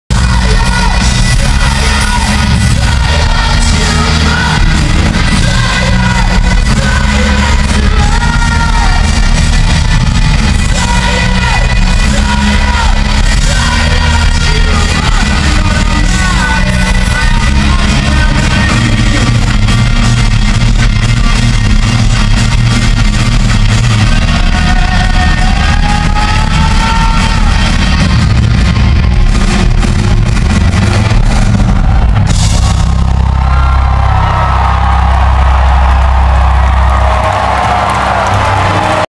30 seconds to mars milwaukee 12-17-09
Alittle bit of the ton of crowd surfers